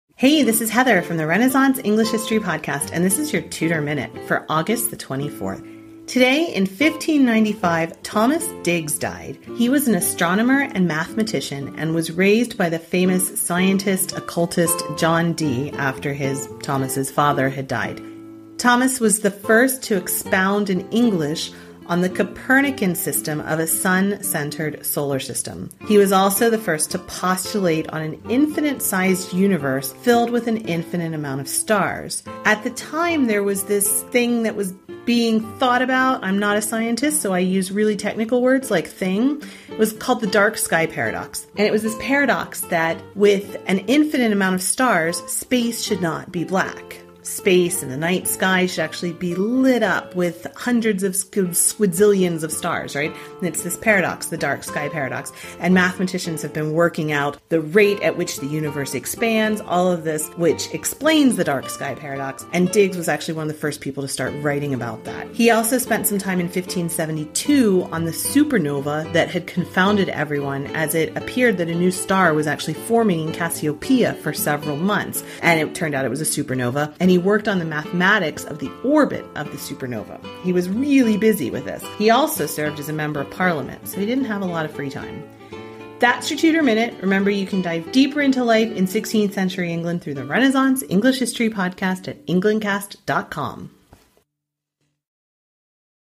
August 24, 1595: Thomas Digges died | Tudor Minute
Today in 1595 Thomas Digges died. He was an astronomer and mathematician, and was raised by the famous scientist/occultist John Dee after his father died.

Suggested link:
Episode 084: Looking up at the sky in Tudor England